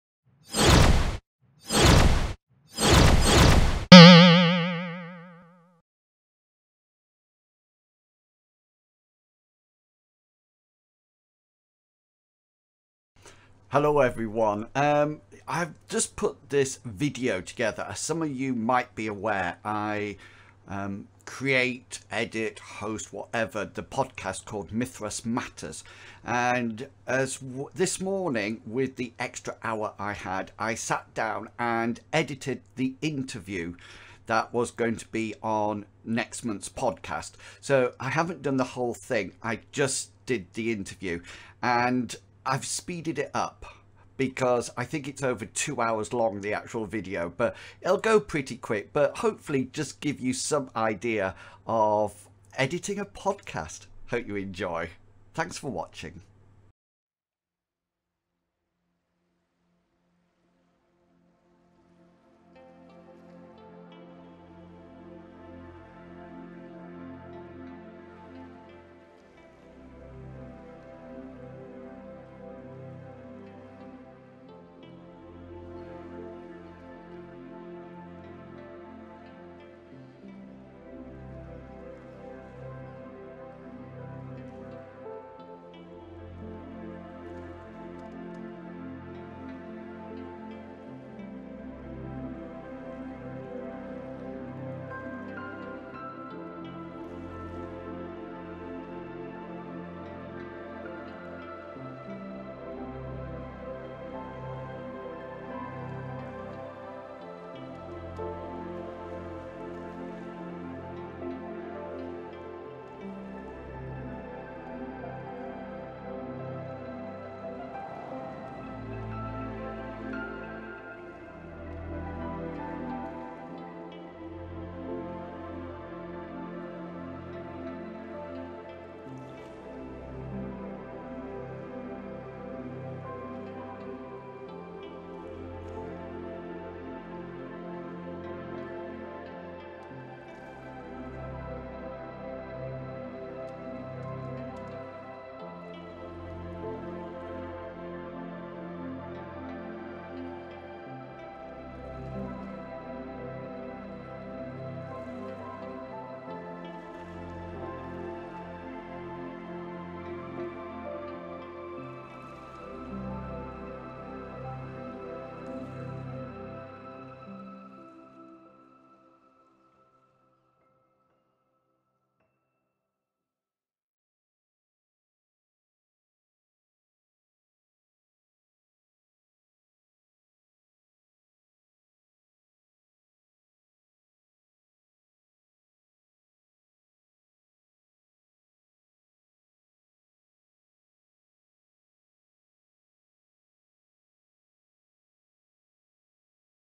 Speed Editing a Podcast
In this video I give you a sneaky peek behinds the scenes as I edit the interview section for the Mythras Matters podcast - three hours of work in just over two minutes!